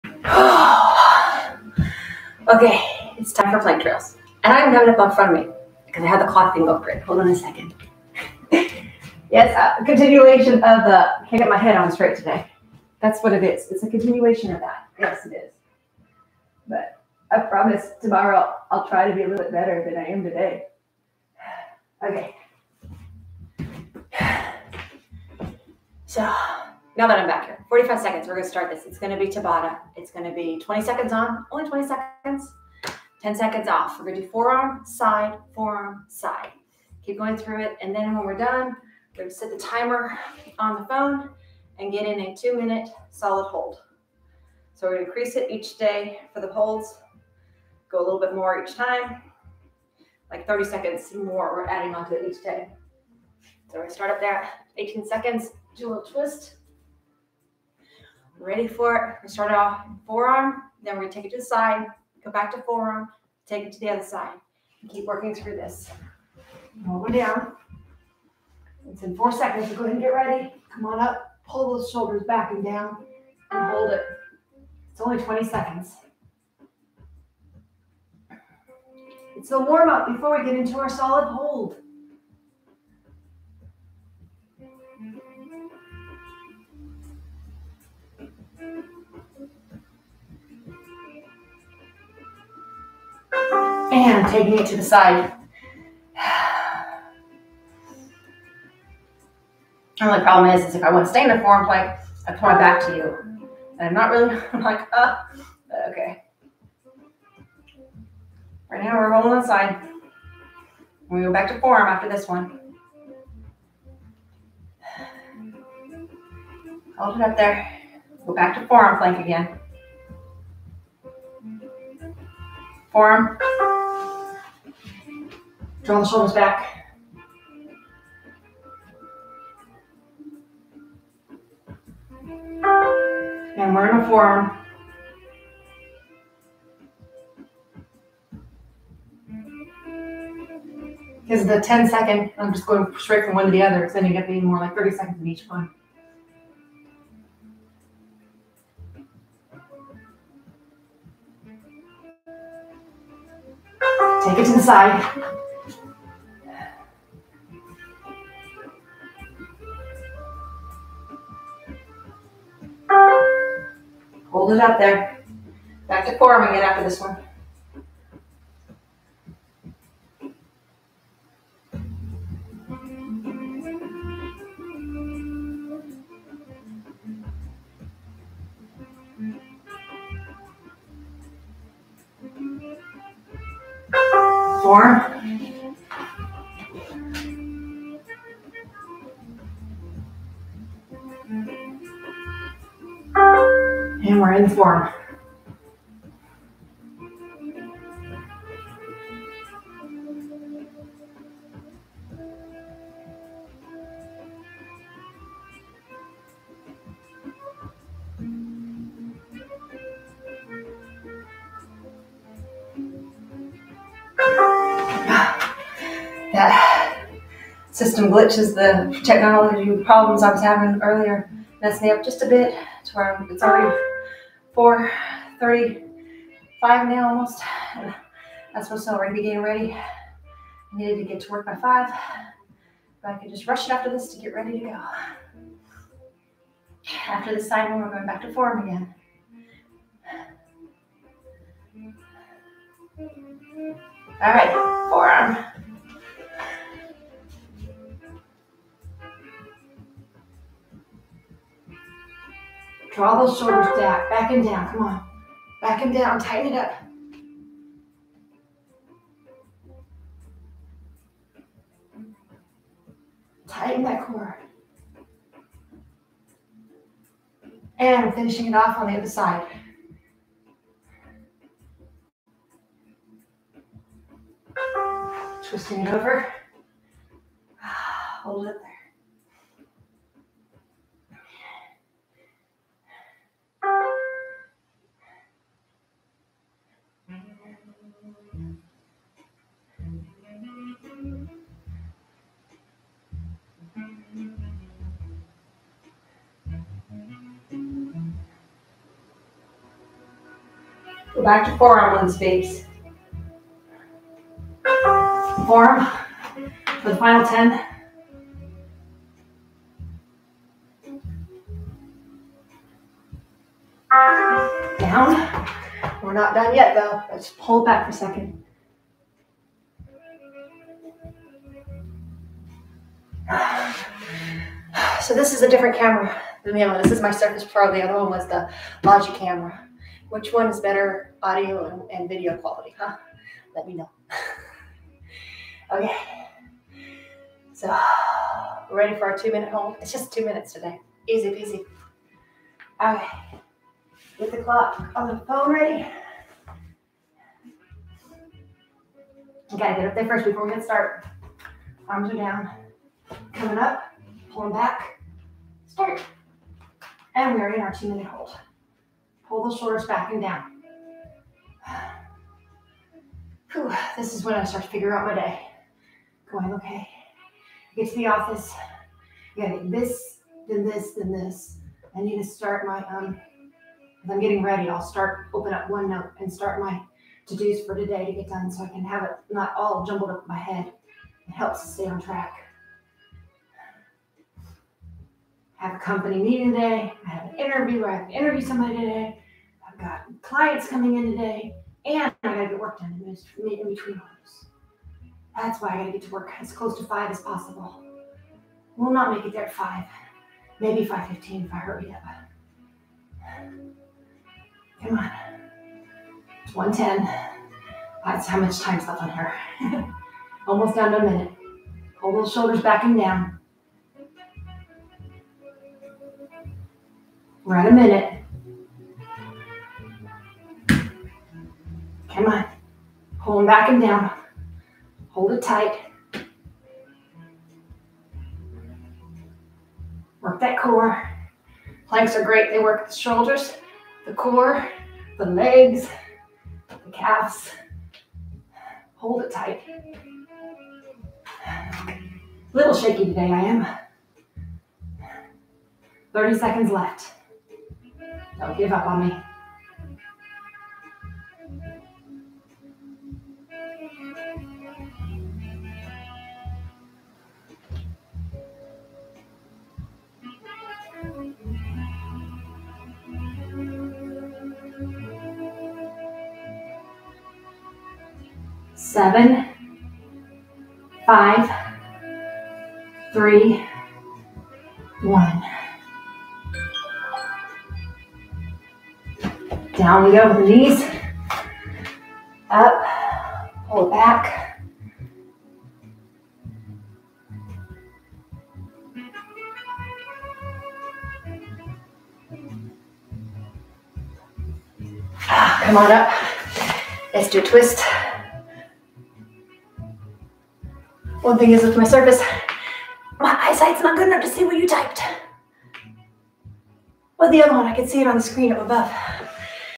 Tabata Plank Fun Week Day1
20 Seconds on 10 Off
        • Forearm
        • Side

Then a 2 Minute Hold